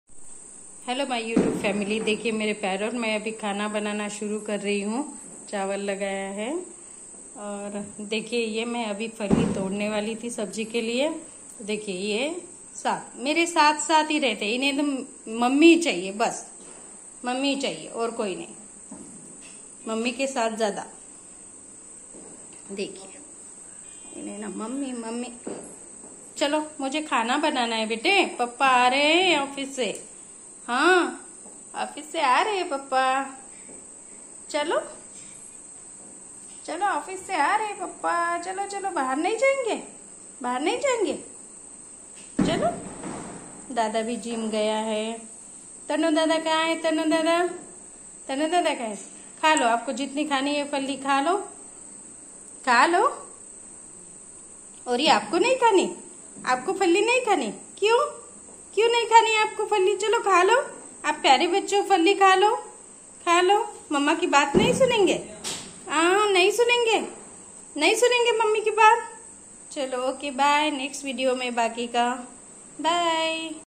🌽🦜🪔 Pyare parrots 🌽 Fali Kha Rahe 💥🌽💕diwali2024
Indian parrot Rio
Fali kha rahe
Funny and cute parrot
Diwali 2024